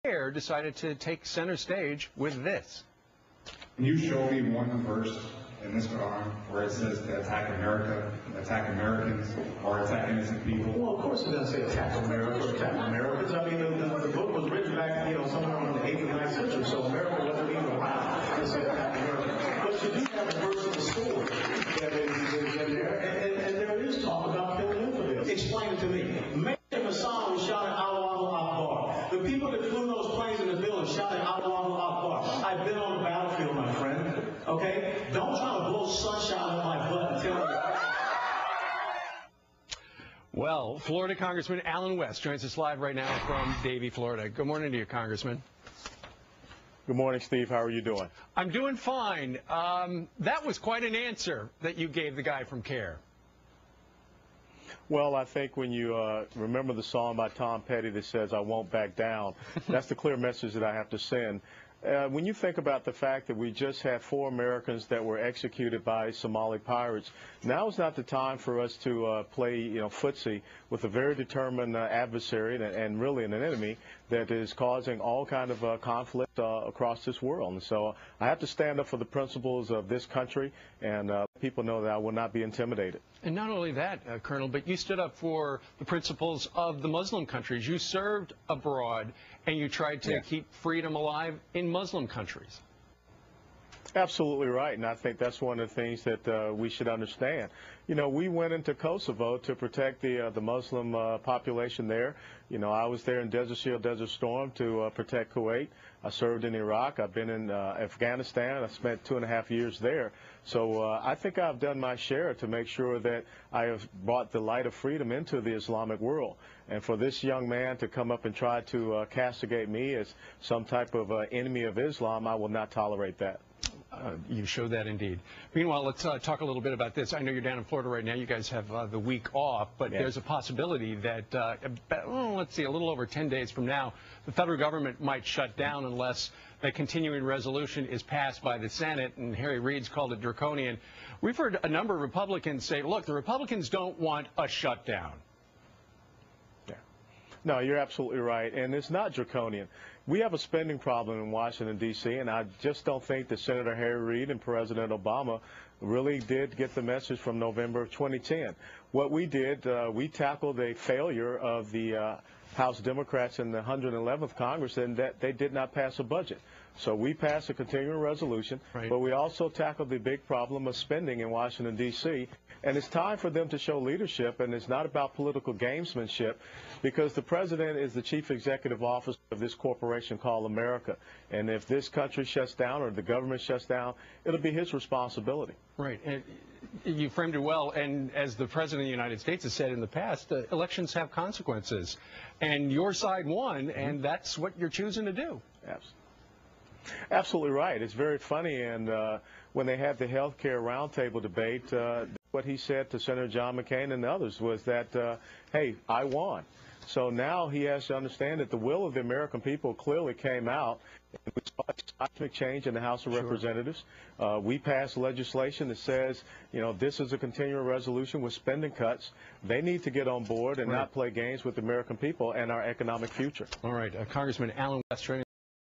"Don't Blow Sunshine..."
Congressman West discusses radical Islam on Fox News